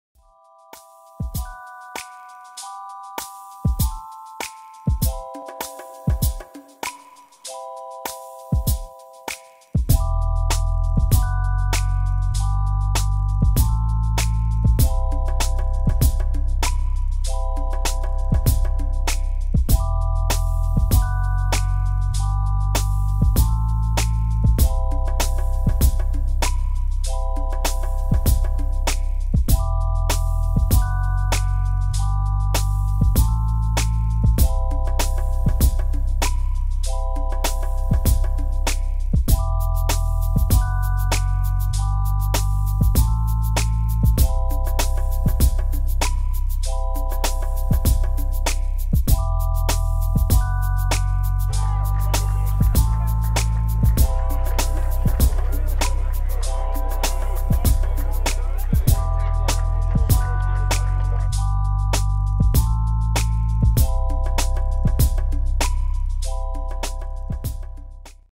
B ROLLIN WITH MARK AVERY MUSIC
TAKE A RIDE WITH MARK AVERY MUSIC AS HE PROVIDES THE SOUNDS THAT FLOW ALONG THE HUDSON.